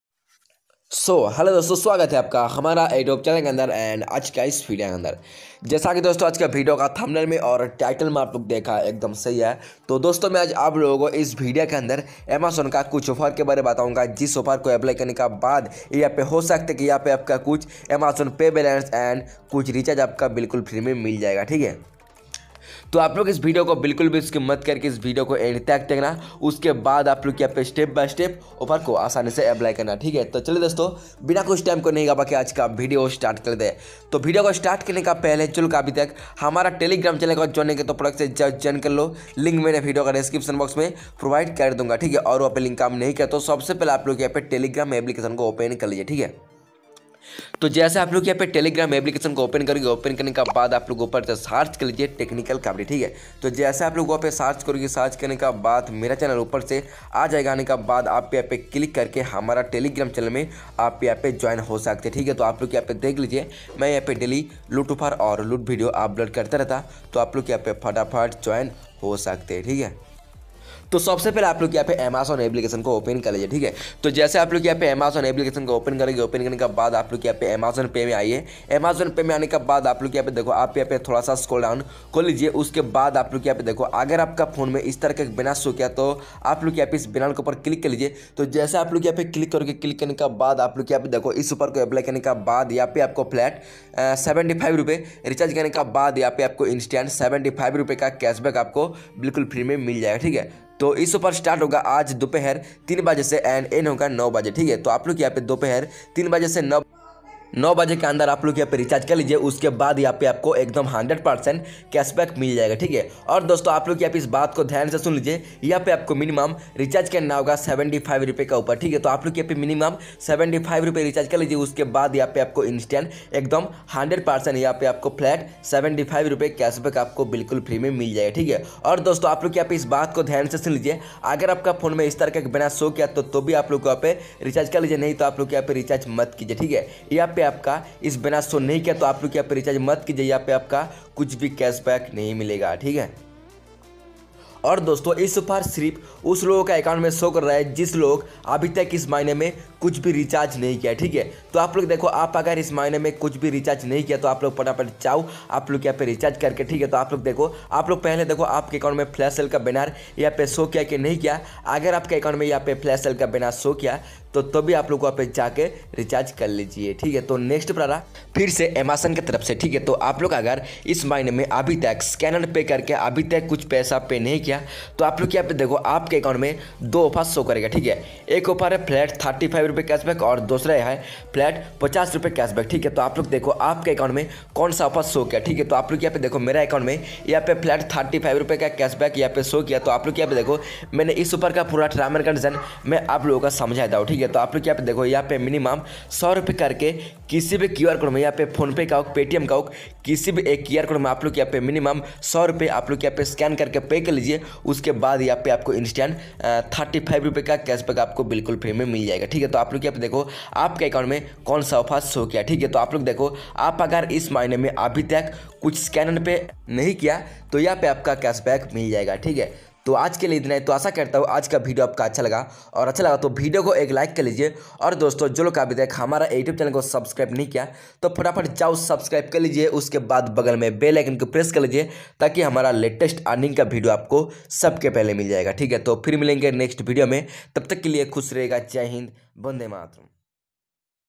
" Today " Amazon Pay ₹75 +₹35 New Offer !! Amazon Free Recharge !! Scan & Pay
1. amazon Send money offer
2. amazon scan and pay offer
3. amazon pay new offer
4. amazon free recharge offer
5. free recharge offer
6. today loot offer

NOTE: THIS VIDEO IS ONLY FOR EDUCATIONAL AND INFORMATIONAL PURPOSES. I Anirban from Technical Kapri channel can't guarantee that you will make money from this or any of my video.